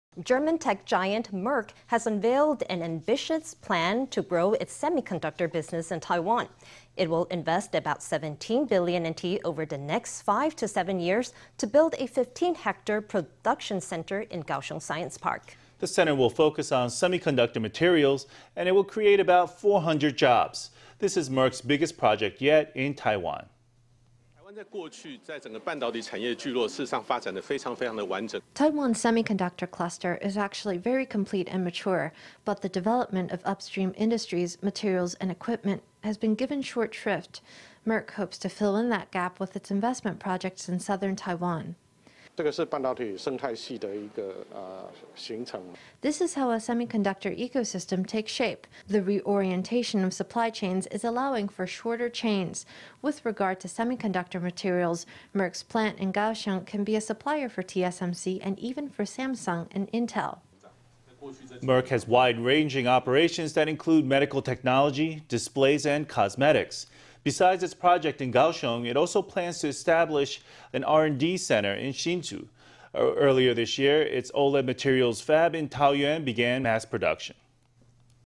Merck announces NT$17 billion production center project in Kaohsiung
German tech giant Merck has unveiled an ambitious plan to grow its semiconductor business in Taiwan. It will invest about NT$17 billion over the next five to seven years, to build a 15-hectare production center in Kaohsiung Science Park. The center will focus on semiconductor materials, and it will create about 400 jobs. This is Merck’s biggest project yet in Taiwan.

John Lee
Merck Taiwan chairman
Taiwan’s semiconductor cluster is actually very complete and mature. But the development of upstream industries, materials and equipment has been given short shrift. Merck hopes to fill in that gap with its investment project in Southern Taiwan.

Chen Chi-mai
Kaohsiung mayor
This is how a semiconductor ecosystem takes shape. The reorientation of supply chains is allowing for shorter chains. With regard to semiconductor materials, Merck’s plant in Kaohsiung can be a supplier for TSMC and even for Samsung and Intel.

Merck has wide-ranging operations that include medical technology, displays and cosmetics. Besides its project in Kaohsiung, it also plans to establish an R&D center in Hsinchu. Earlier this year, its OLED materials fab in Taoyuan began mass production.